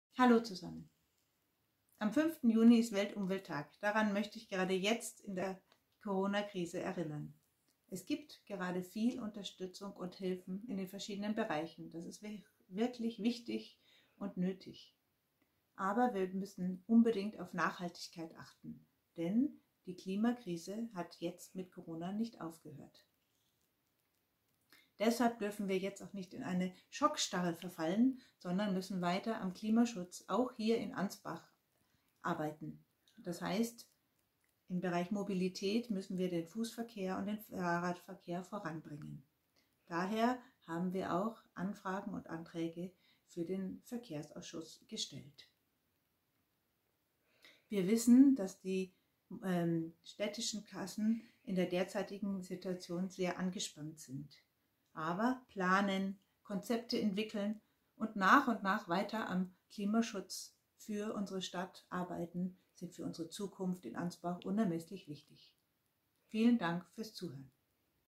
Statement zum Tag der Umwelt
Meike Erbguth-Feldner, Fraktionsvorsitzende der Grünen im Ansbacher Stadtrat